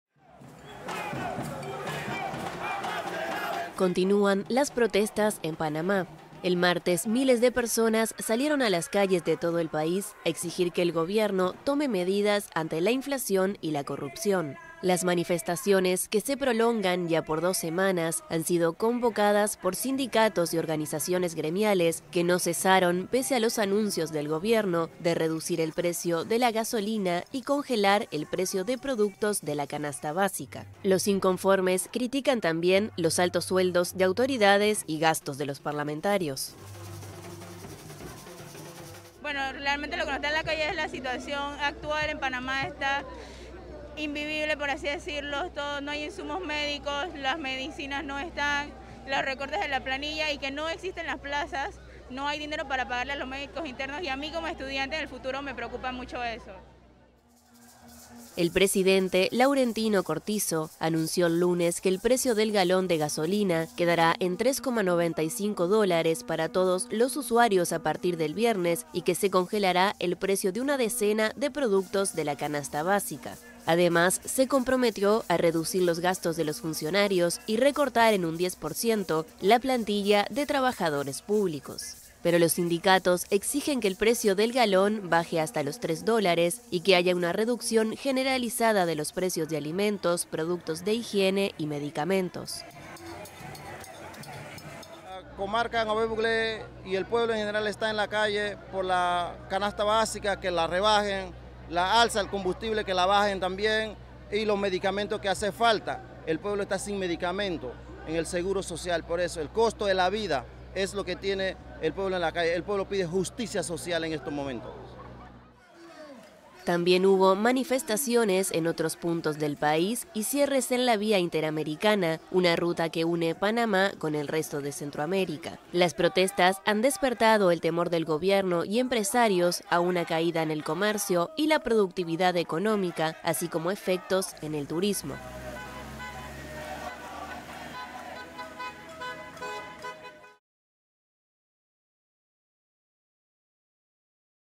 PROTESTAS EN PANAMÁ pese al anuncio de bajada de combustibles y alimentos | EXPLICADO
Miles de personas protestaron nuevamente en Panamá para exigir al gobierno medidas contra la inflación y la corrupción, pese al anuncio del presidente panameño, Laurentino Cortizo, de bajar el precio de los combustibles y algunos alimentos.